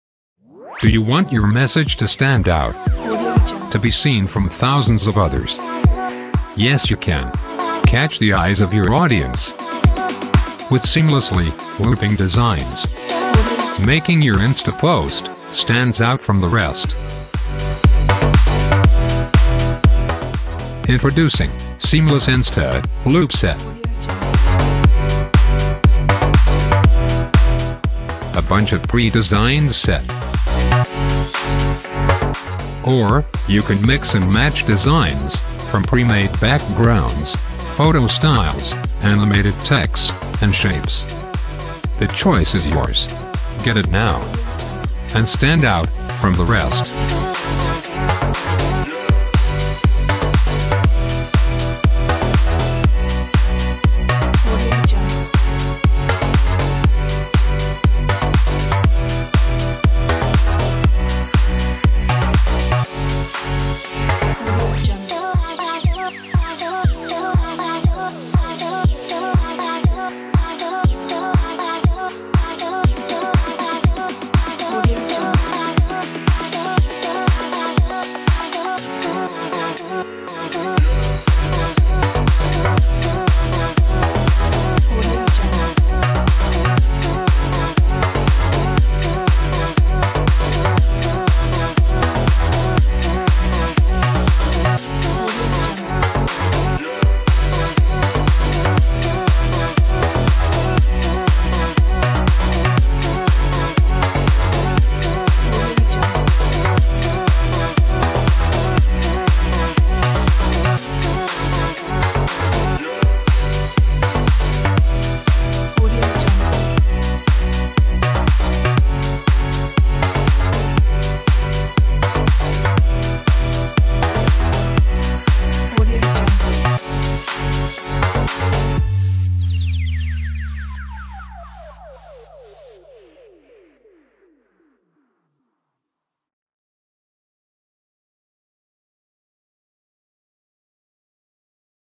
Insta Seamless Loop Set 3
Instagram Posting to promote your instagram advertisment, profile, people, friends and more. Completely organized and very easy to customize! Just add icon images or videos to your placeholder icon and adjust the color and text color. All colors and texts are customisable.